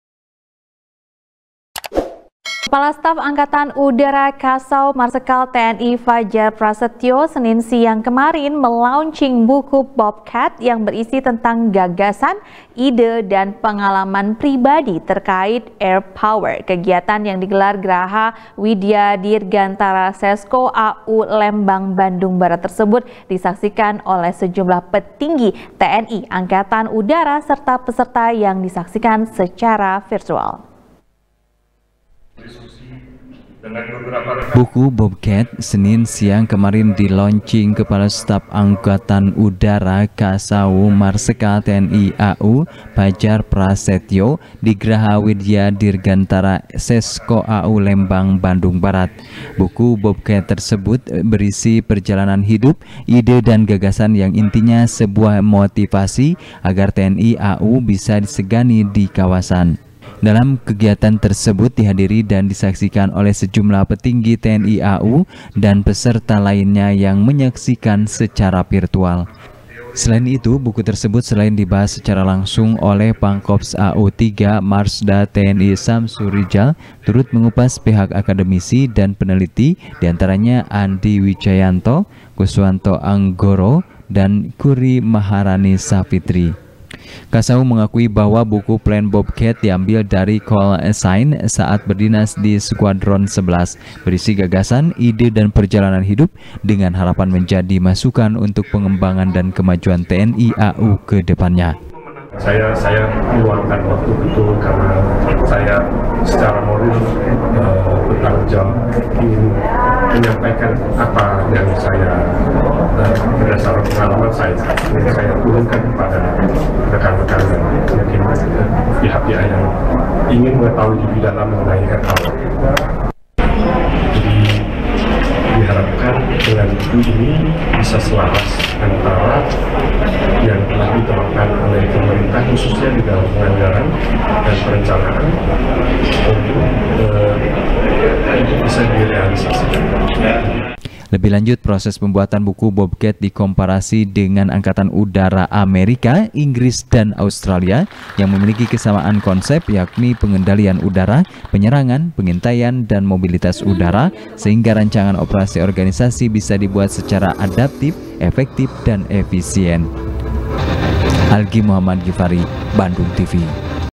Kepala Staf Angkatan Udara Launching Buku Plan Bobcat | SBR | BANDUNGTV
SBR | BANDUNGTV | KRISANTI | Kepala Staf Angkatan Udara, Kasau , Marsekal Tni Fadjar Prasetyo, Senin Siang Kemarin ,Melaunchng Buku Bobcat,Yang Berisi Tentang Gagasan,Ide,Dan Pengalaman Pribadi,Terkait Air Power. Kegiatan Yang Digelar Graha Widya Dirgantara,Seskoau Lembang, Bandung Barat Tersebut, Disaksikan Oleh Sejumlah Petinggi Tni Au, Serta Peserta Yang Disaksikan Secara Virtual.